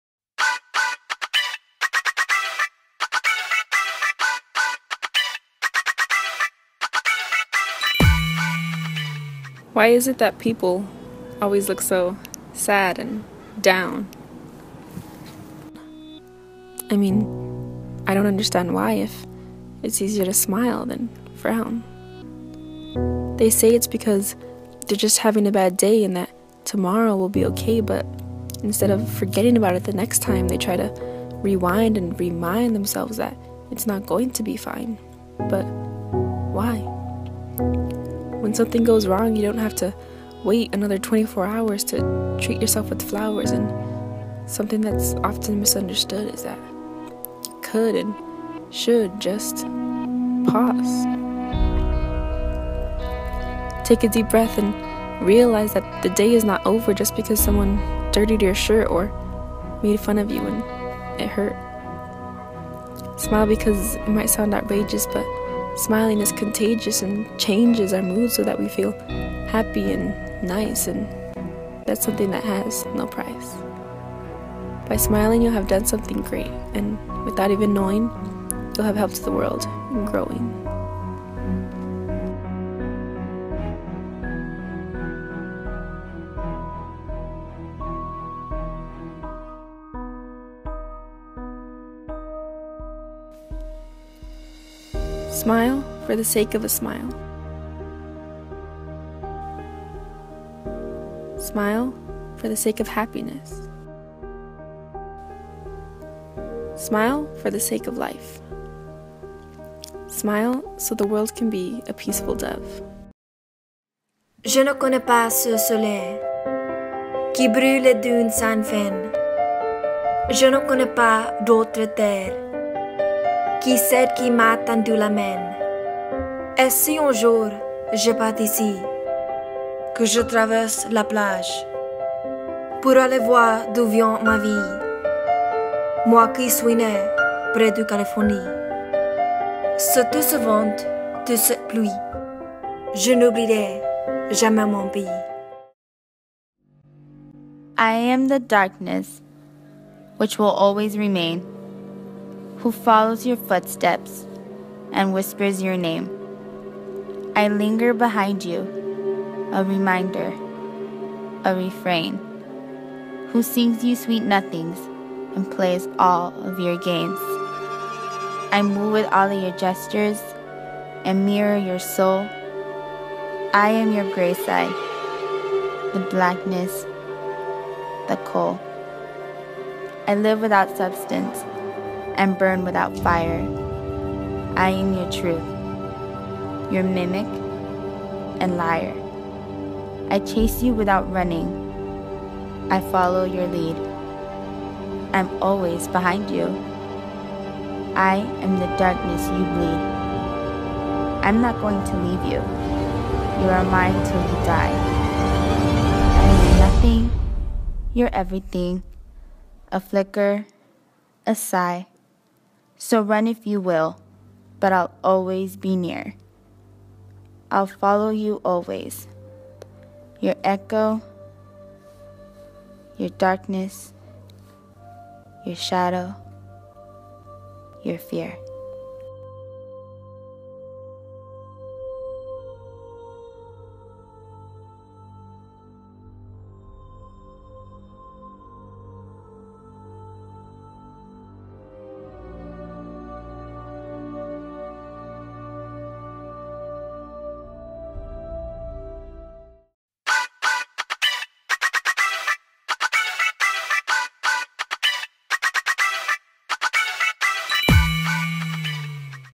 2016 Nominees - Best Video Poem
For this assignment, I asked my students to produce a video poem.  First, they were to record a voice over for either a preexisting or original poem.  Then, they were asked to shoot a short video to accompany their poem and use the voice over as the narration of their story.

These particular short films have been nominated for a special end-of-the-year honor, to be named at the CSHM fine arts banquet in May, 2016.